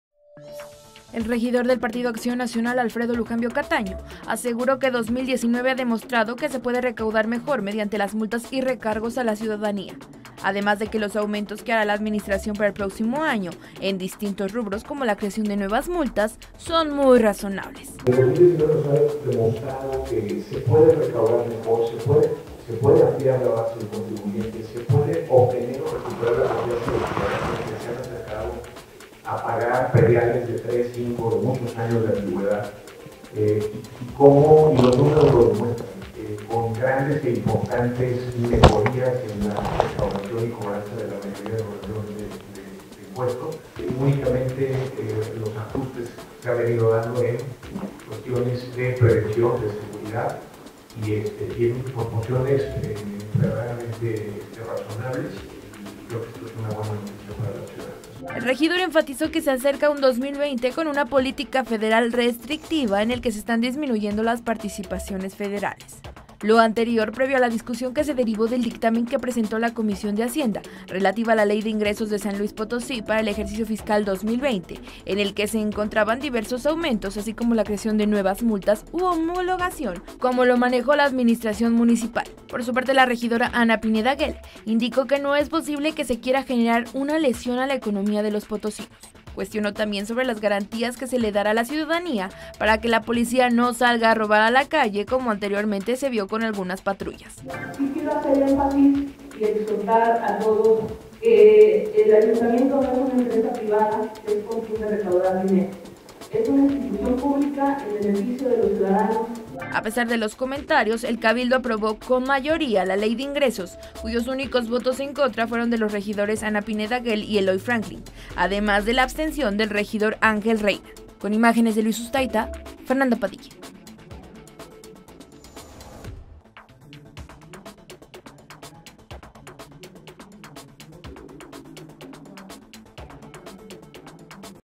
"2019 nos ha demostrado que se puede recaudar mejor, aumentos son razonables": Regidor panista
Se aprobó por mayoría la Ley de Ingresos en el Cabildo capitalino.